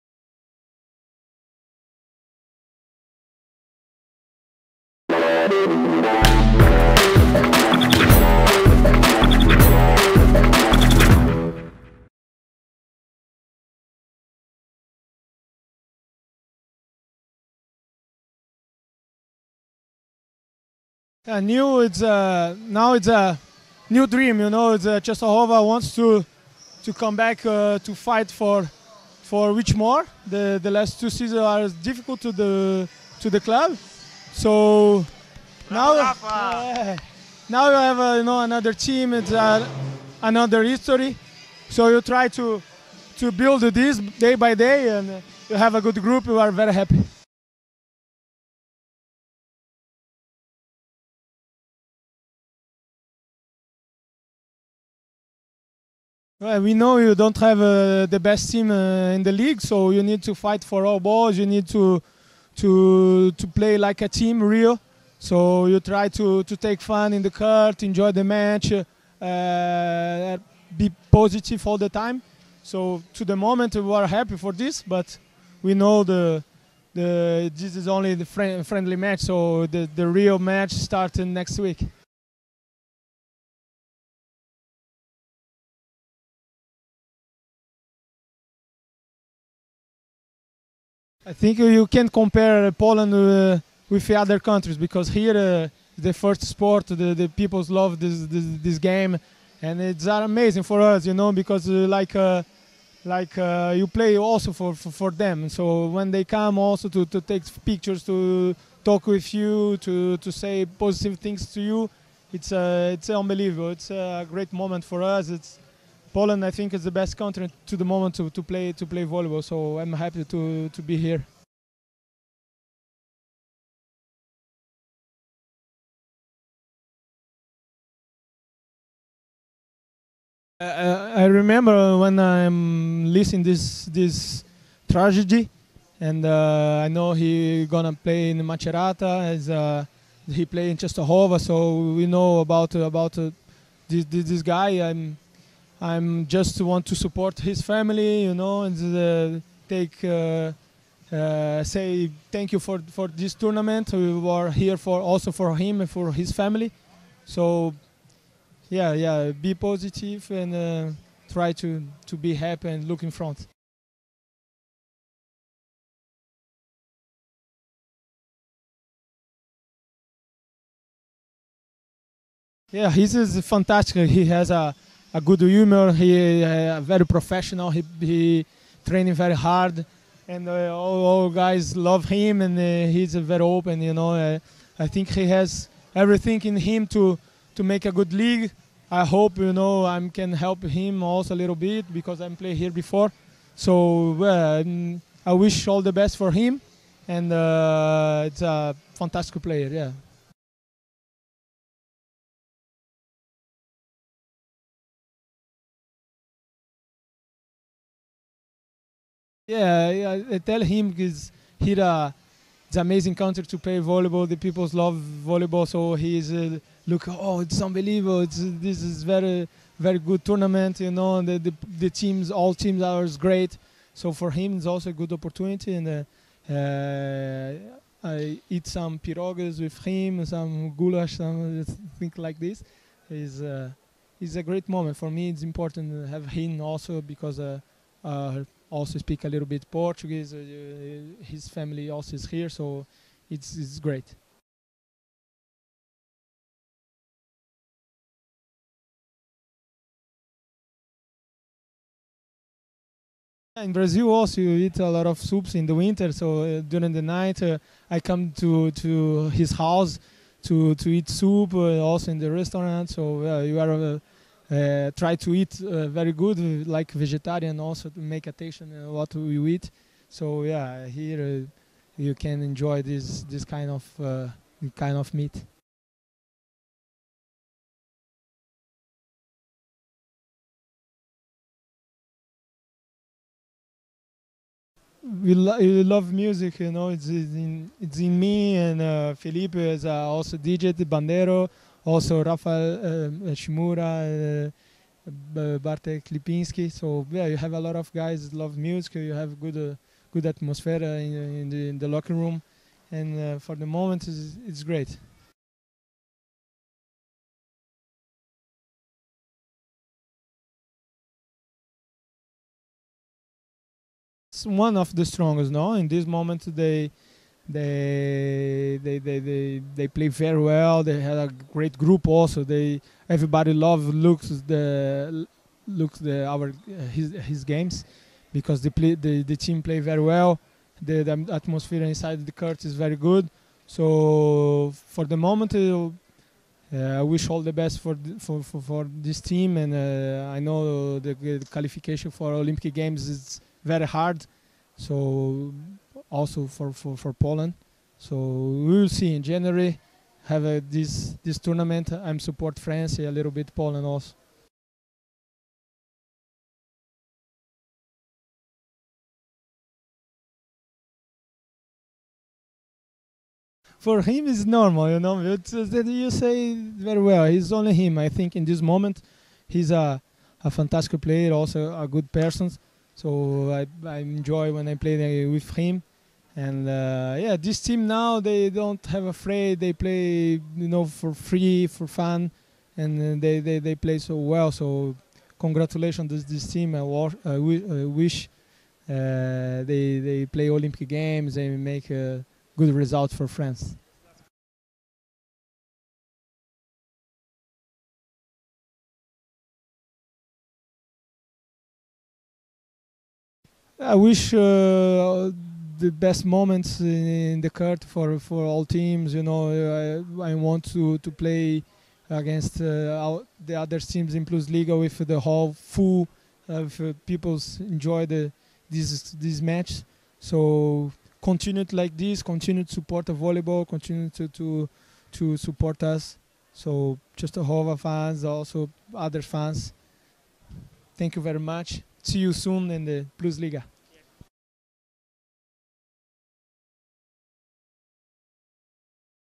Rafael Redwitz dla siatkowka24.com: W szatni mamy kilku Dj'ów!
Zapraszamy do oglądania wywiadu z rozgrywającym AZS-u Częstochowa - Rafaelem Redwitzem, nie tylko o siatkówce, ale także Arkadiuszu Gołasiu, polskiej kuchni oraz DJ'u w szatni ekipy spod Jasnej Góry.